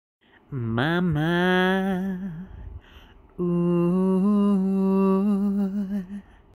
Sing it for Mama! Put your Heart and Soul into it!
If only musical masterpieces could be performed by adorable baby apes. That would be majestic!